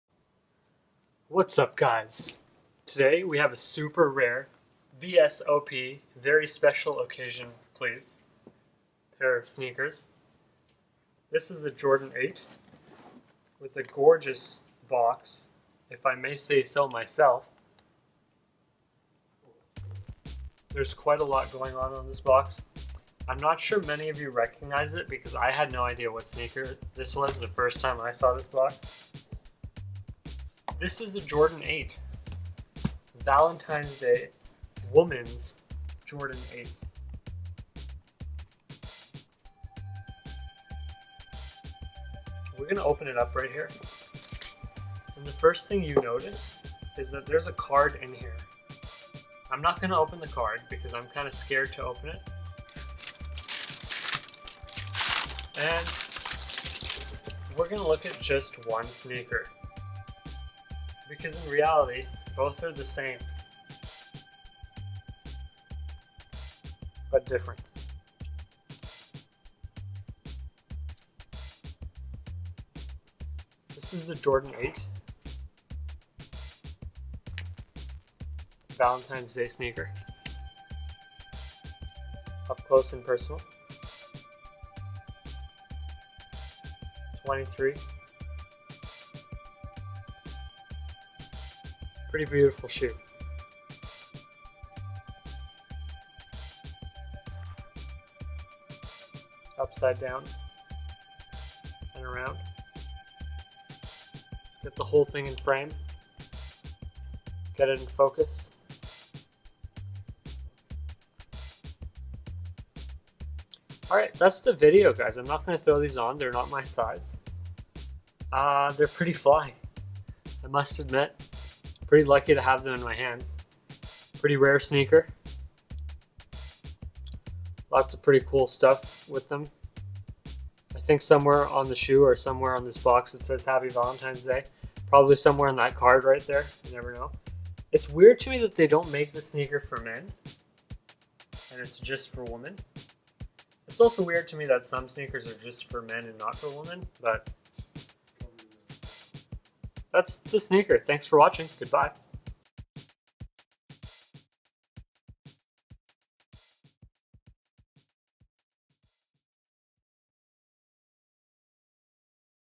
Nike AIR JORDAN 8 VDAY VALENTINES DAY Jordans Sneaker Unboxing
What do you think of these relatively limited Jordan 8's?